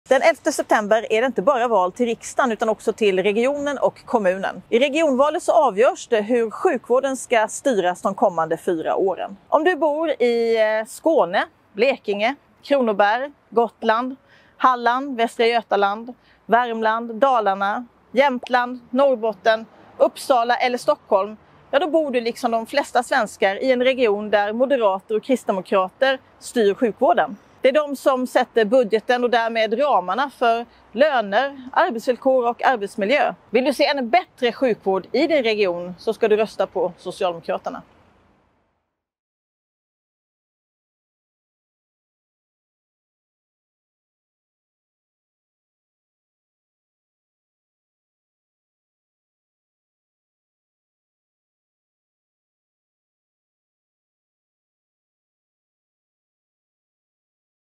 Vet du vem som styr sjukvården?
I många regioner går sjukvården på knäna. Personalen är utsliten och väntetiderna för människor som behöver vård är ofta långa. Det är inte värdigt Sverige. Idag bor 8 av 10 svenskar i regioner där Moderaterna och Kristdemokraterna styr sjukvården. Vill du se en bättre sjukvård i din region? Då ska du rösta på Socialdemokraterna i regionvalet.

Vi Socialdemokrater vill ta tillbaka kontrollen över sjukvården. Vi vill anställa fler sjuksköterskor, fler läkare och förbättra arbetsvillkoren för all vårdpersonal. Vårdköerna ska kortas så du får den hjälp du behöver, när du behöver den.
Vi vill också göra upp med den borgerliga marknadsstyrningen av vården. Det är en svår strid men vi tar den, eftersom i ett starkt samhälle ska rätten till en förstklassig sjukvård, oavsett plånbok, kön eller bostadsort, vara en självklarhet.